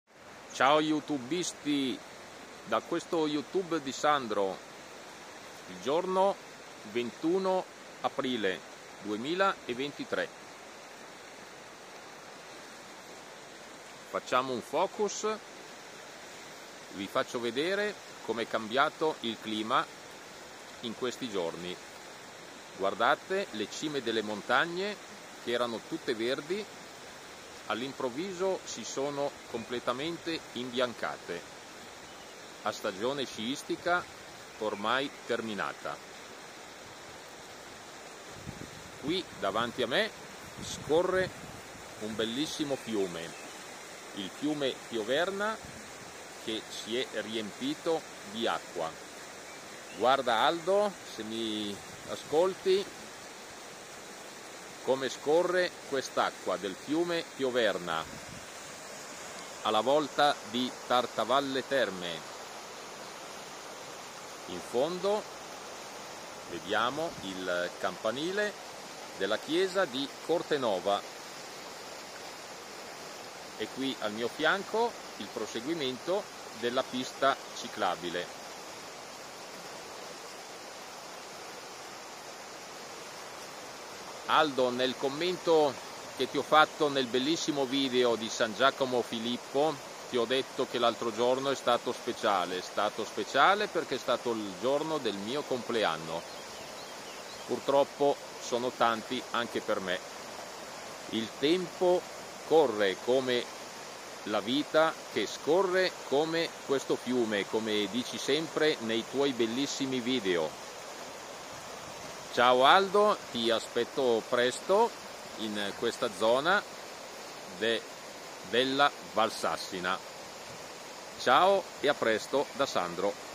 Un clima quasi invernale....con i monti innevati
Lungo la pista dell'altro verso Cortenova. paese della villa De Vecchi